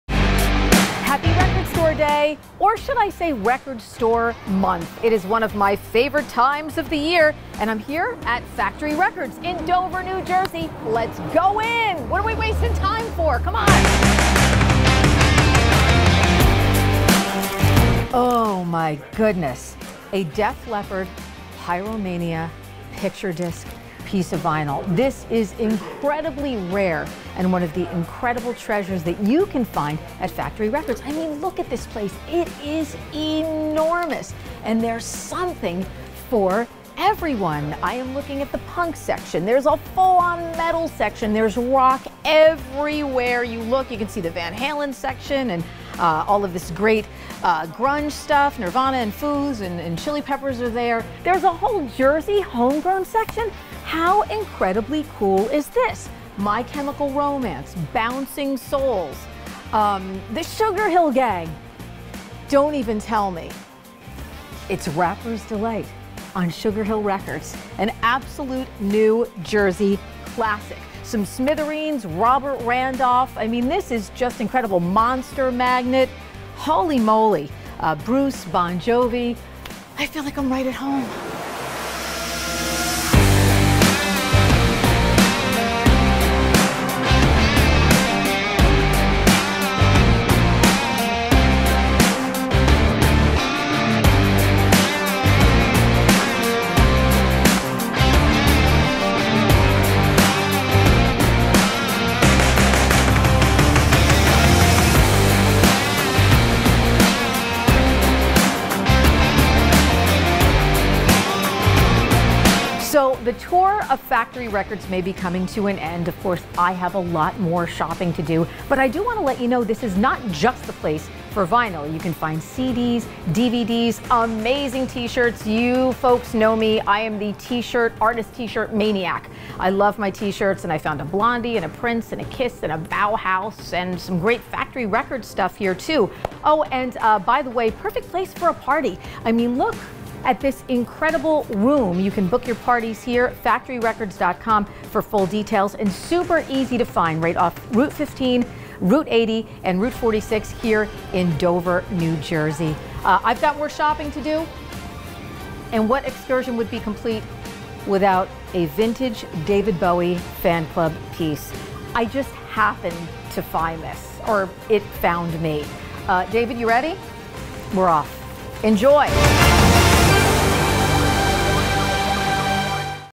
WDHA's Terrie Carr Visits Factory Records In Dover, NJ
105.5 WDHA's Terrie Carr visited the immense Factory Records in Dover, NJ. Albums, CDs, cassettes, DVDs, etc. They have everything, including a great lounge/performance space that can be rented. It's the perfect place to visit for Record Store Day.